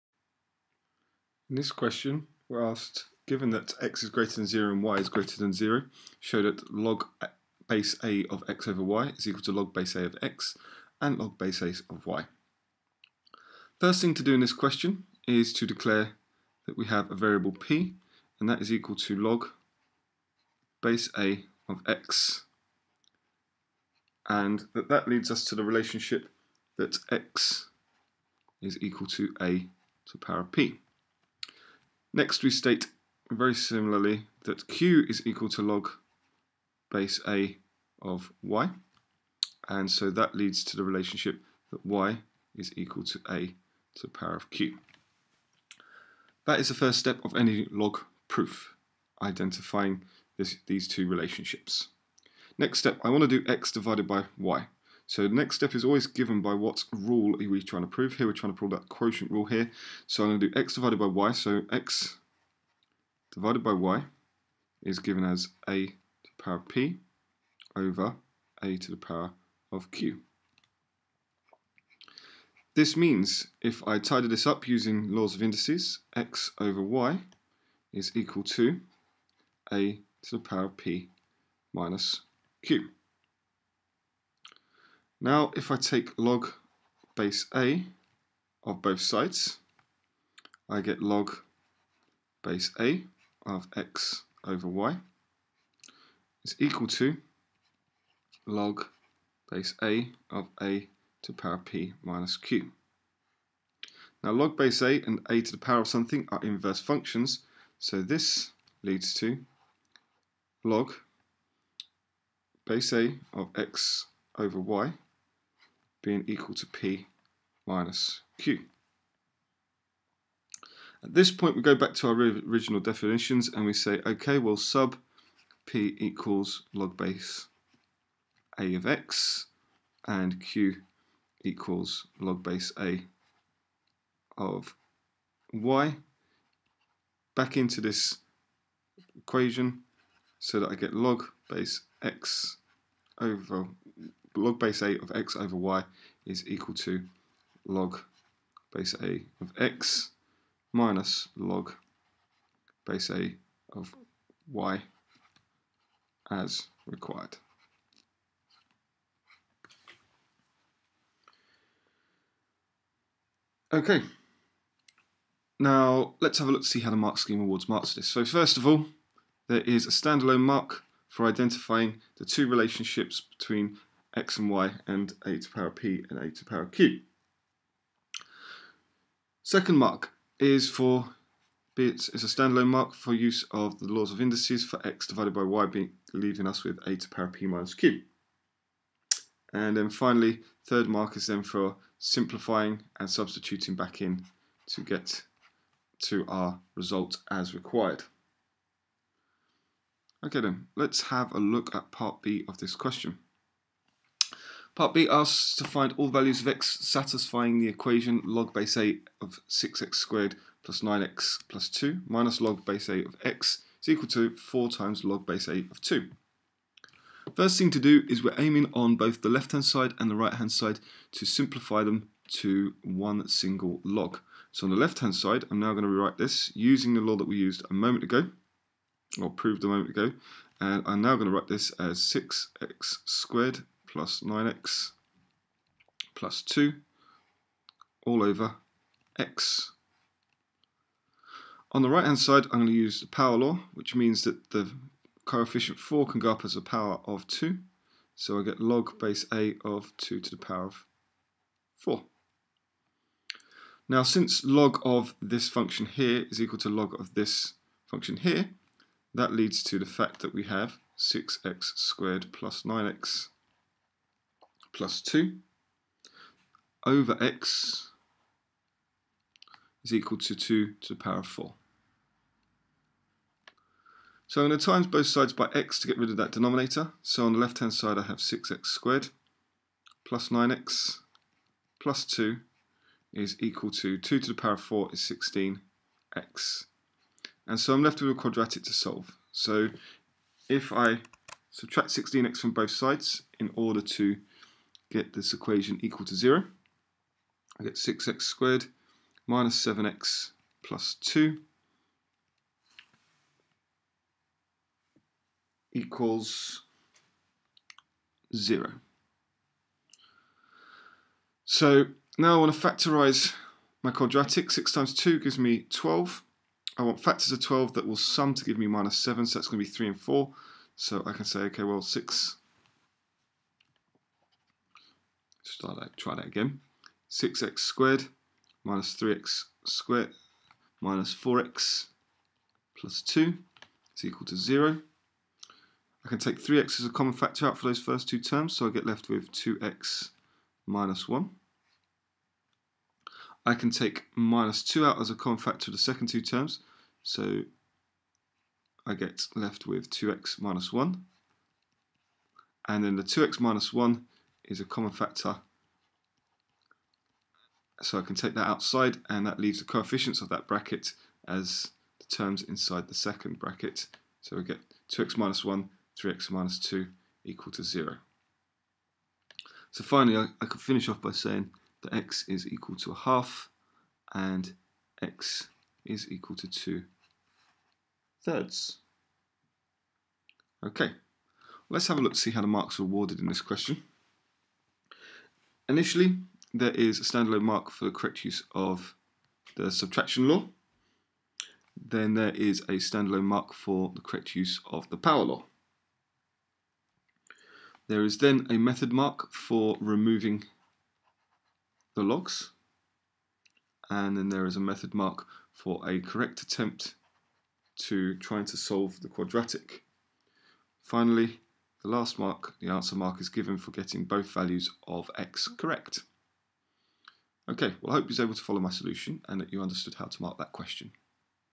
WJEC C2 June 2015 Q7 - Logarithms Solution
Solution and mark scheme for the WJEC C2 June 2015 exam paper.